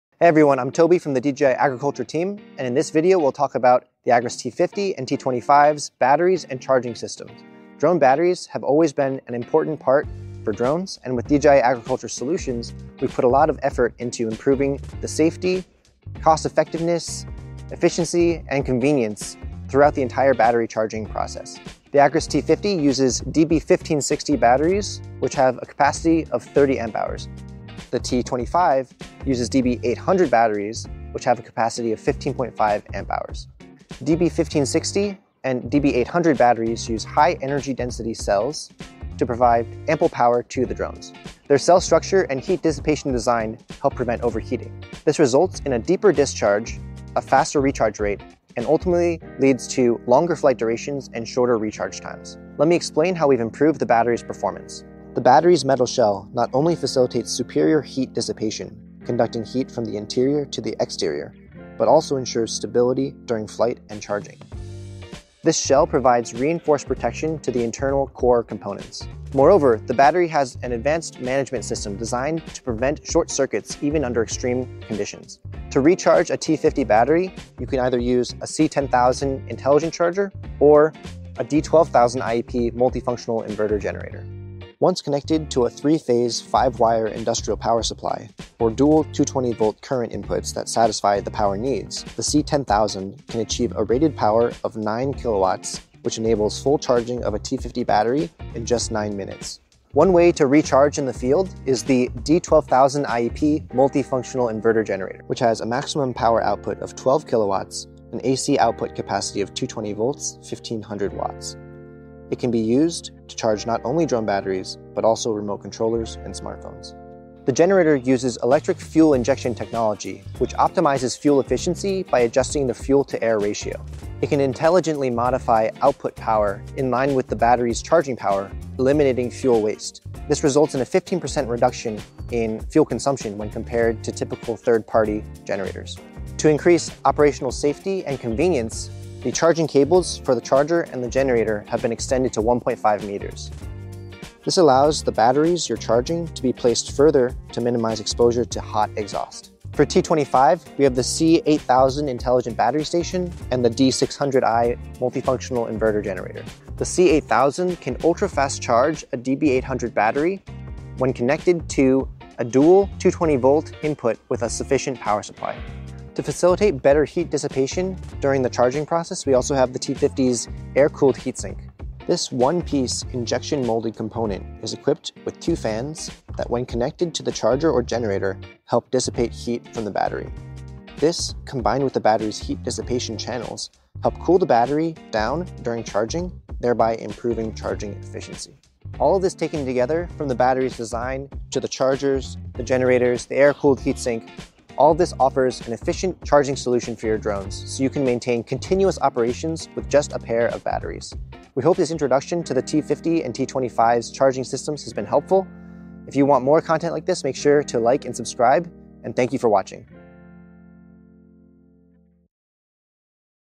Características de las baterías y del cargador para los DJI Agras T50 y T25 - DJI ARS MADRID
Las características de las baterías y de los cargadores de los DJI Agras T50 y T25